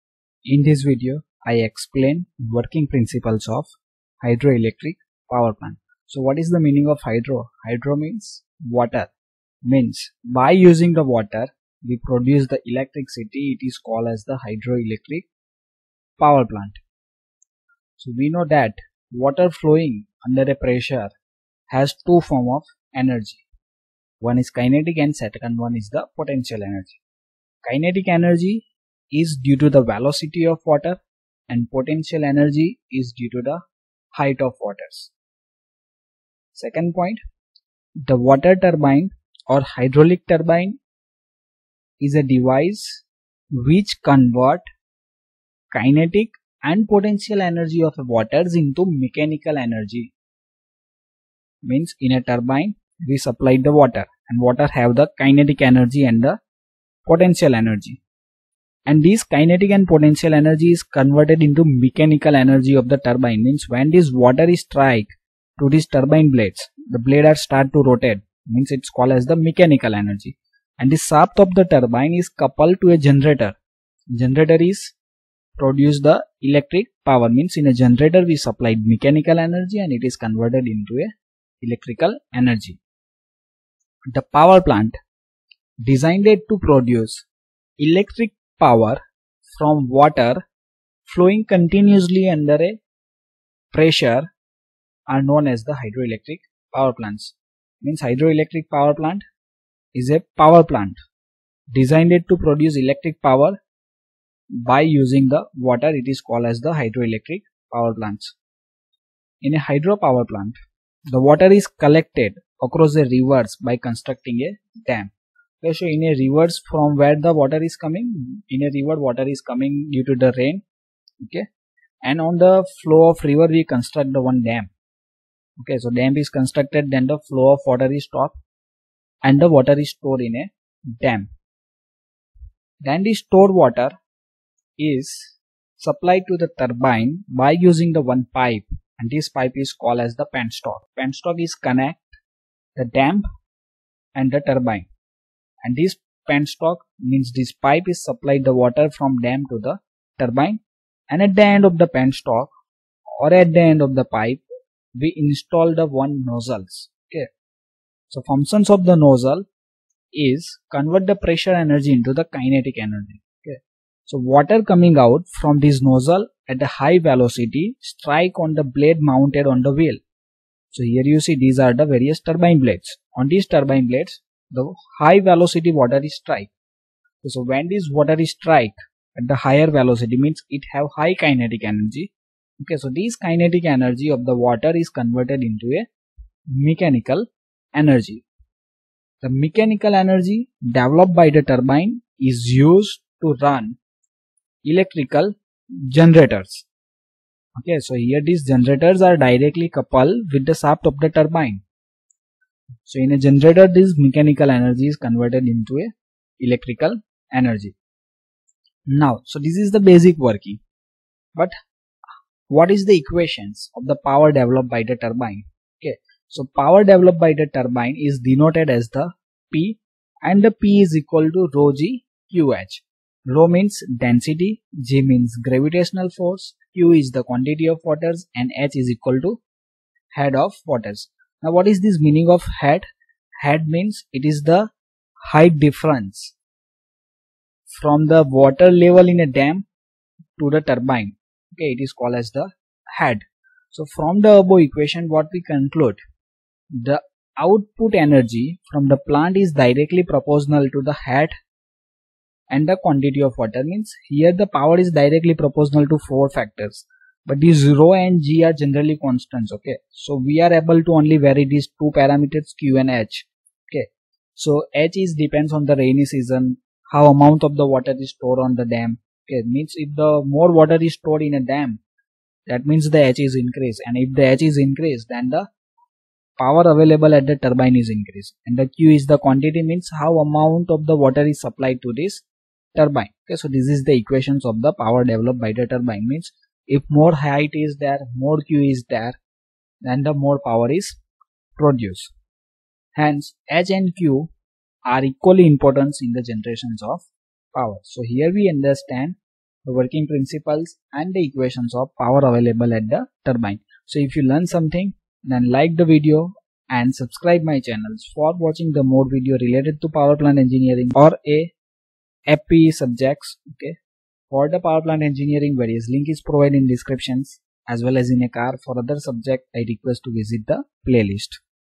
Working Principle of Hydro Electric Power Plant
In this video, I explained Working Principle of Hydro Electric Power Plant.